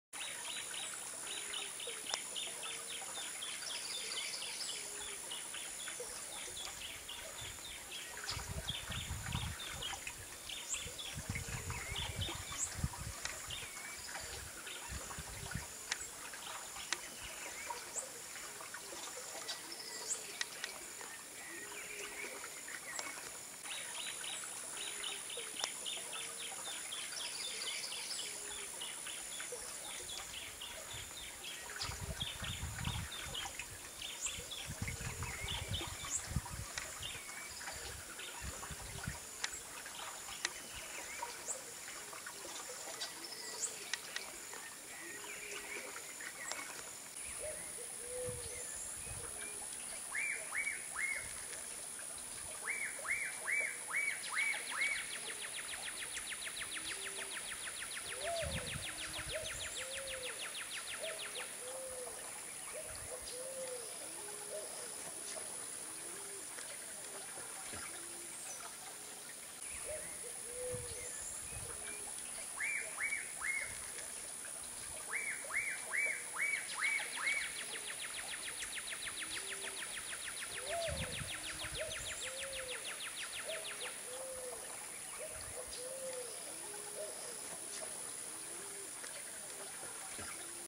CABEÇA-SECA, CABEÇA-DE-PEDRA, (MYCTERIA AMERICANA), WOOD STORK, Ave pescadora.
Cabeça-seca. Nome Científico: Mycteria americana, Nome em Inglês: Wood Stork. Classe: Aves, Ordem: Ciconiiformes, Família: Ciconiidae. Conhecido também por: Passarão, Cabeça-de-pedra,  Jaburu-moleque, Trepa-moleque (MT), João-grande (RS) e Padre. Alimentação: Peixes, anfíbios, pequenos répteis e invertebrados. Distribuição Geográfica: Ocorre em todo o Brasil, principalmente no Pantanal. Sua distribuição no Continente Americano se estende do Sul dos Estados Unidos à Argentina.